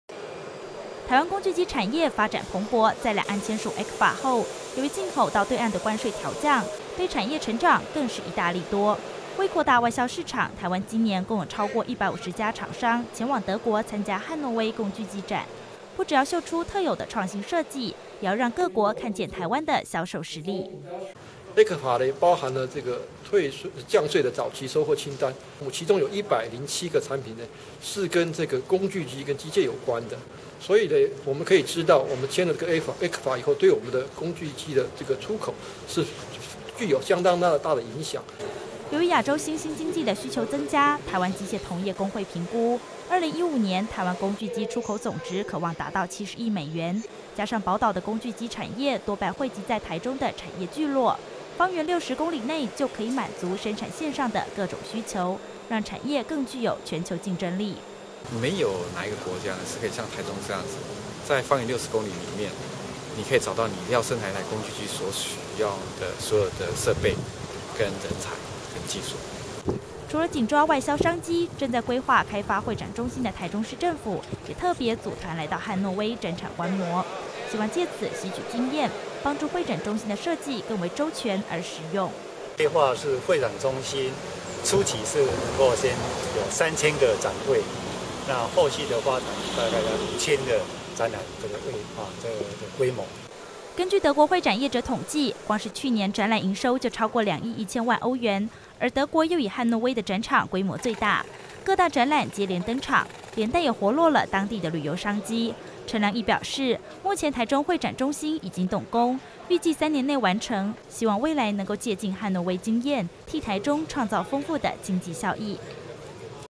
台中市開發會展中心赴德國漢諾威取經
Taichung International Exhibition and Convention Center organized a group to EMO Hanover in Germany.